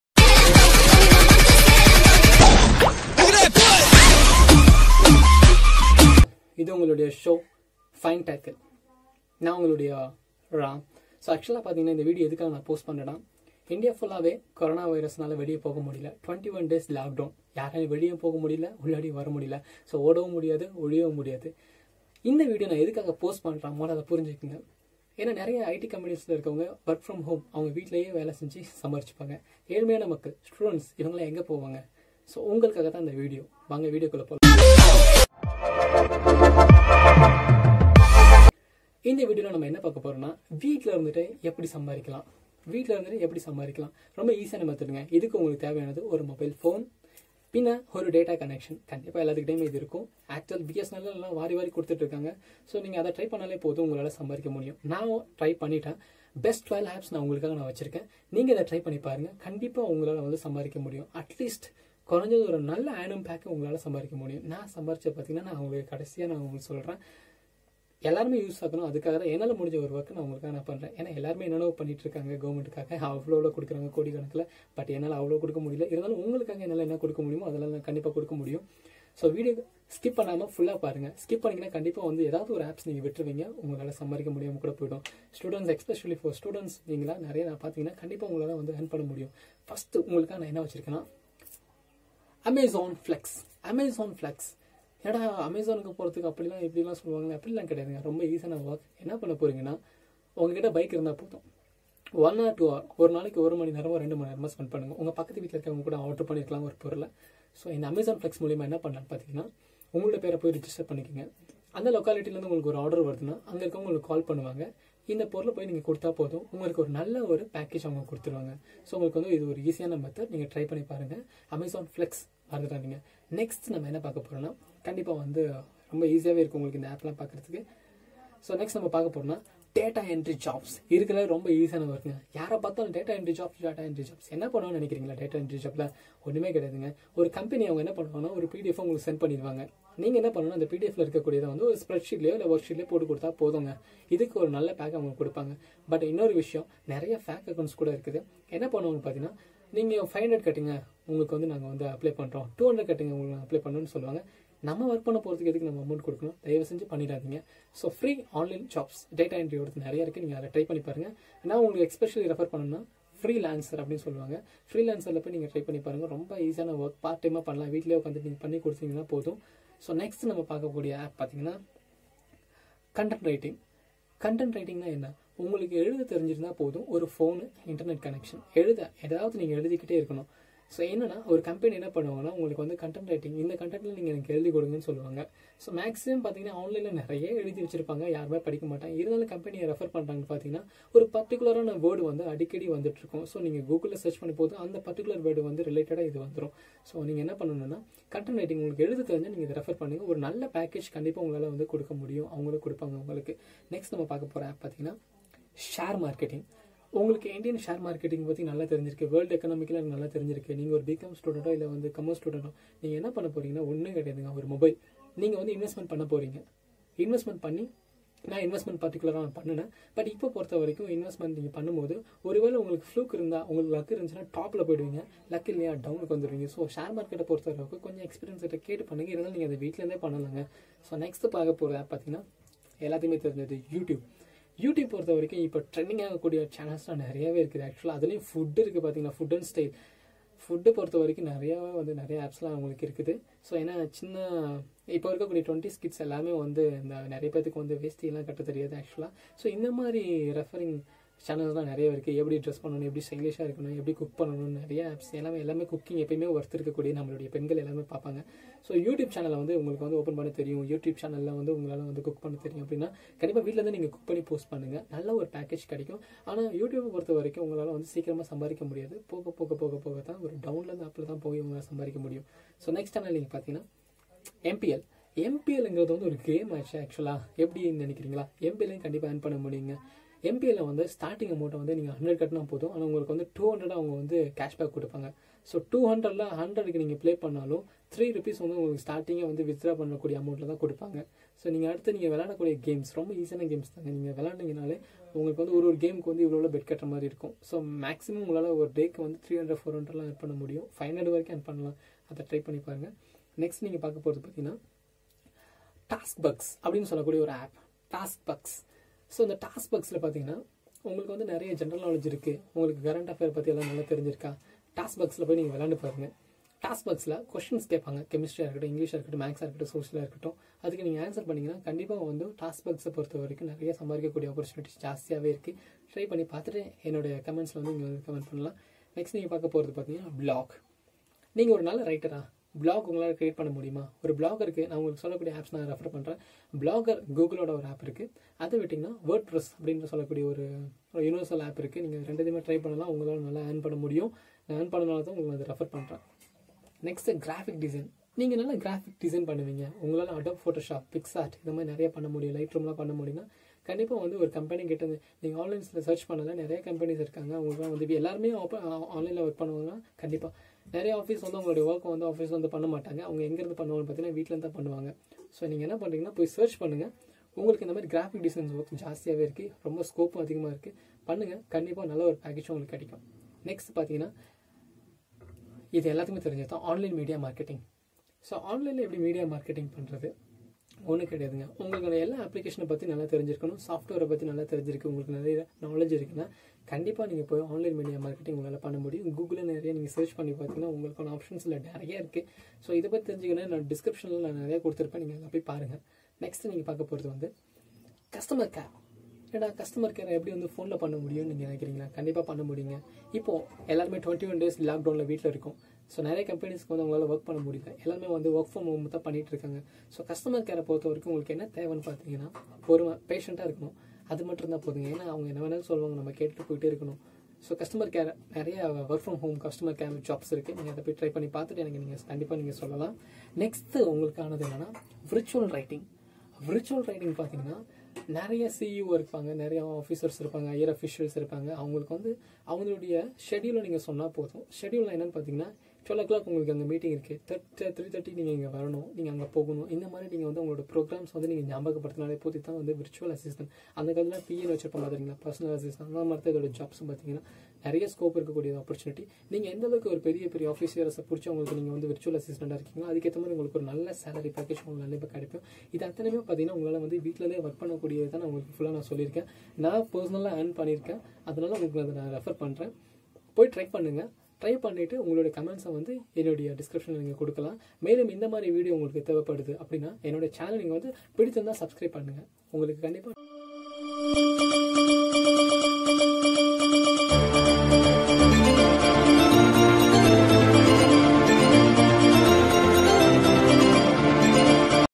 Top free online Jobs/ work from home /trusted /வாங்க சம்பாதிக்கலாம் Daily earnings 500+/
Hello respectives
Welcome to#findtackle channel.
 100% true.

         Ellarukum sambadhika asai erukum so endha channel vungaluku kandipa use agum
So video parunga. Start from today.

Listed sites.
amazon flex
Data entry (freelancer)
Content writing
blog creating (word press, blogger)
Customer Care Service
social media marketing
virtual assistant
task bugs
mpl pro
Graphic design
share marketing.